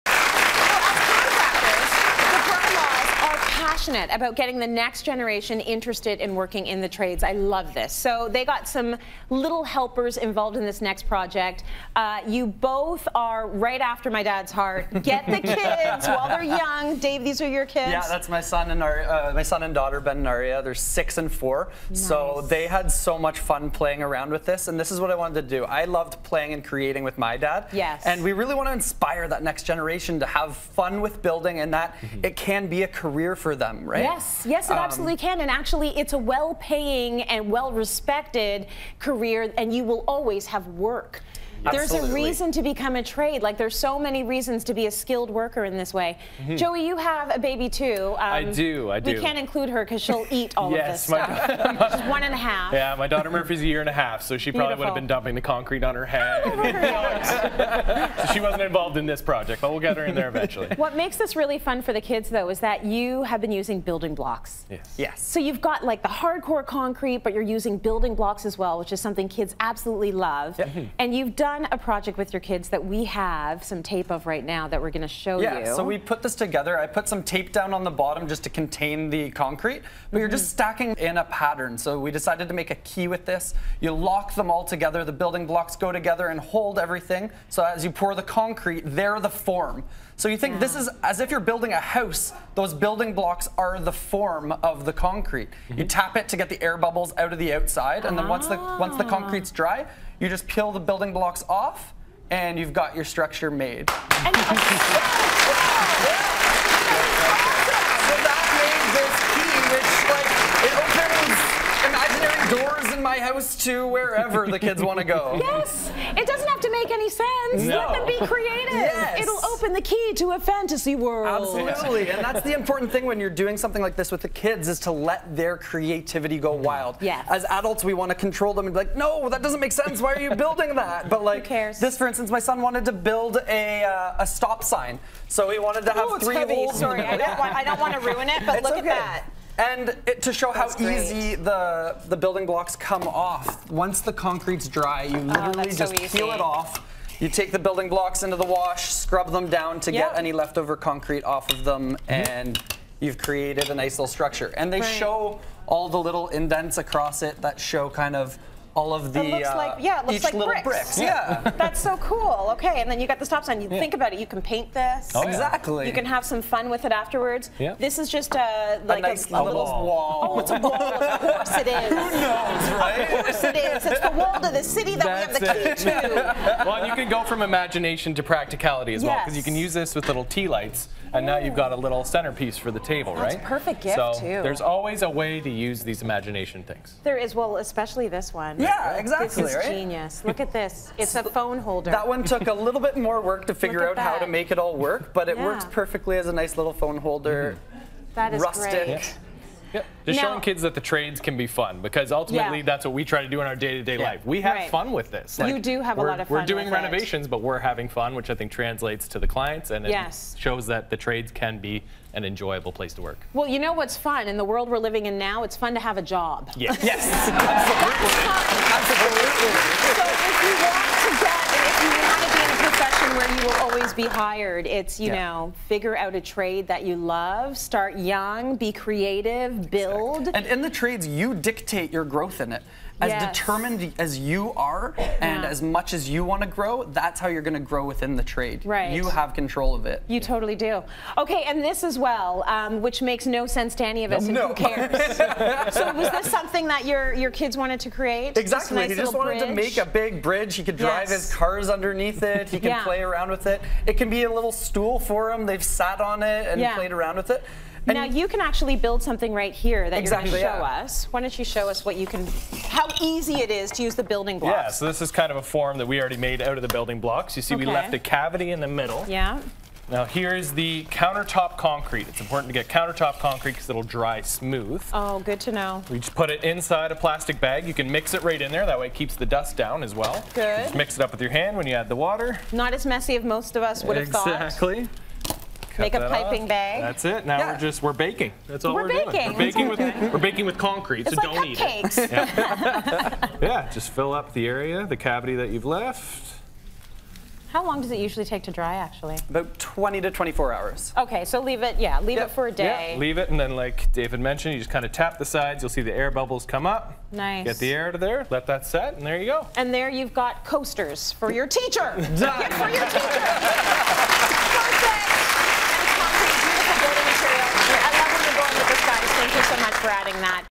How to teach your children through building activities
The BroLaws are passionate about getting the new generation interested in working in the trades. The BroLaws loved playing and creating with their dad and want to inspire the next generation to have fun with building.